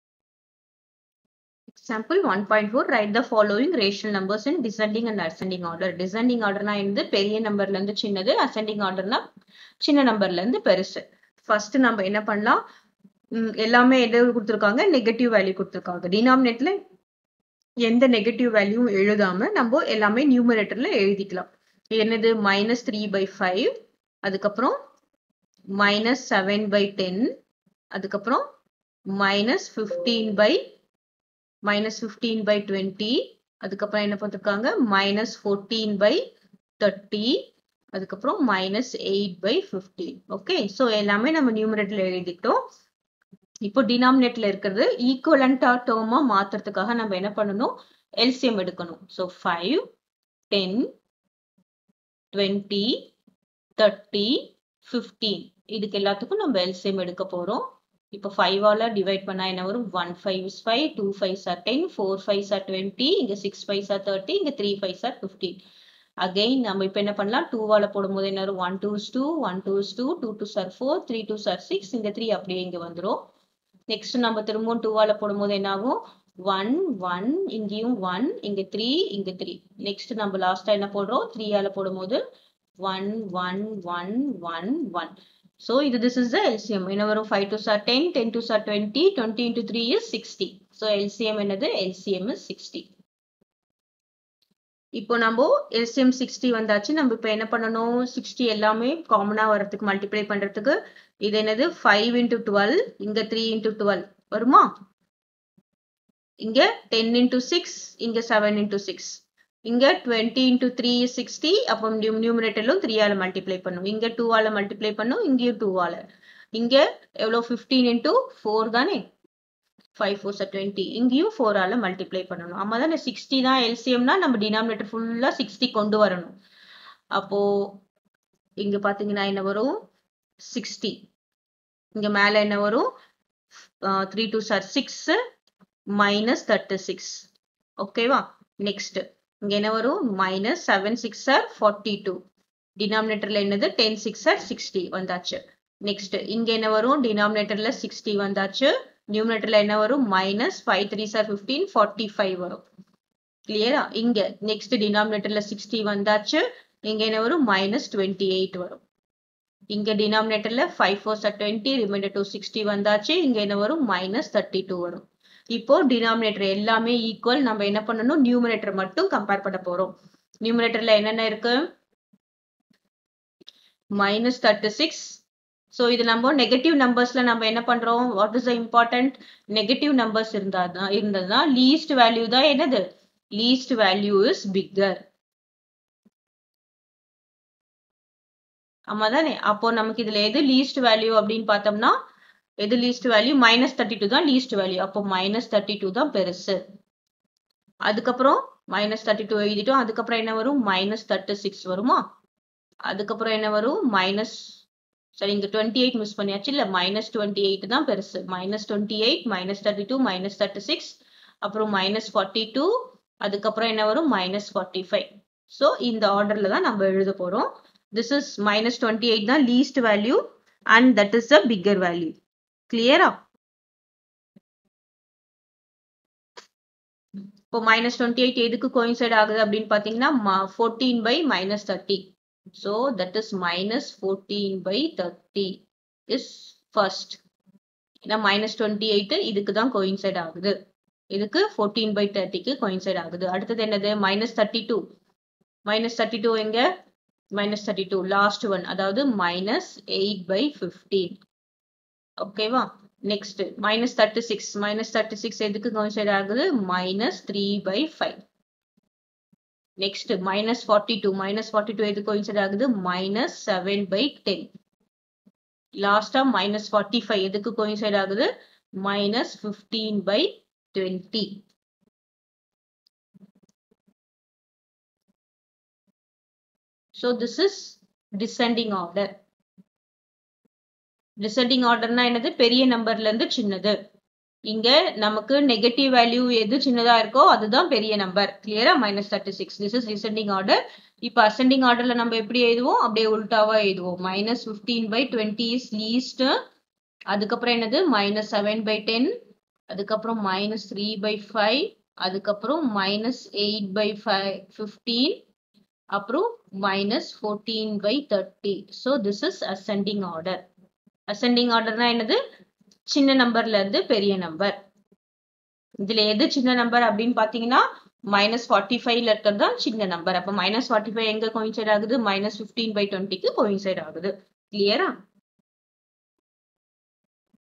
Class 8 TN Maths 1. RATIONAL NUMBERS Example1.4 Write the following rational numbers in descending
TN TERM I 1. RATIONAL NUMBERS
Example 1.4
Write the following rational numbers in descending and ascending order.
-3/7, 7/-10 , -15/20, 14/-30, -8/15